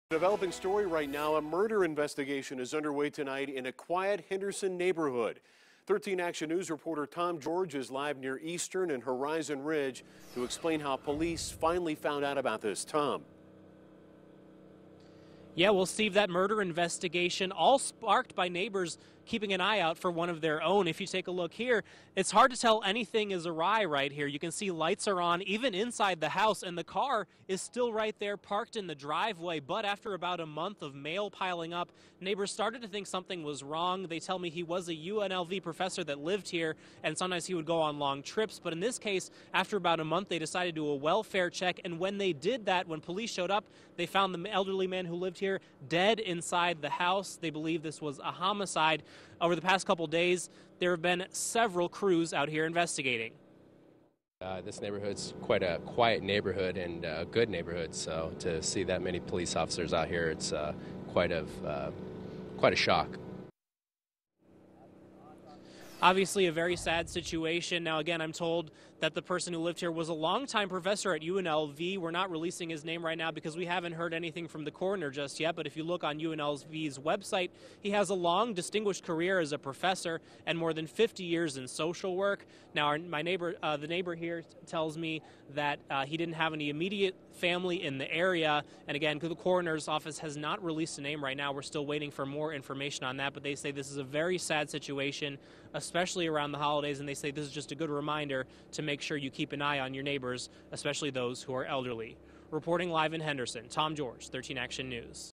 Elderly man found dead inside Henderson home
Neighbors requested a welfare check after no one had seen or heard from him since early November. He was in his mid-70s.